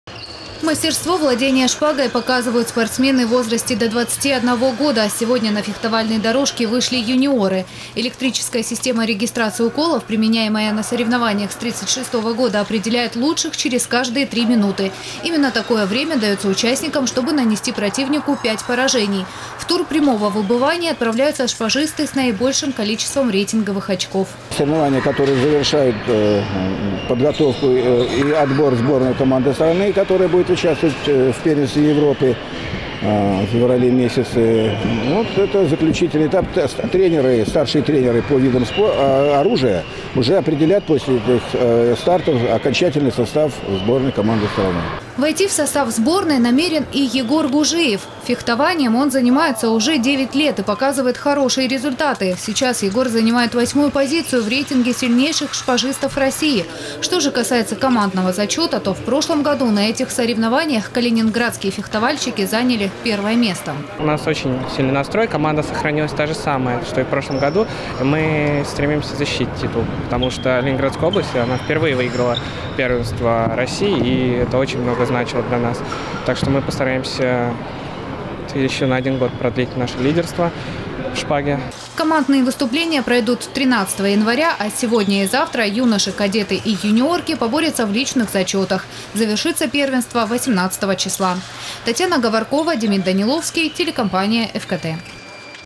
Юные мастера острого оружия собрались в Сочи. Новости Сочи Эфкате
Сегодня в центре единоборств «Юг Спорт» стартовало первенство России по фехтованию. Около сотни спортсменов из 17 регионов страны поборются не только за рейтинговые очки, но и право войти в состав сборной команды.
Сочи. Новости.
Телеканал Эфкате.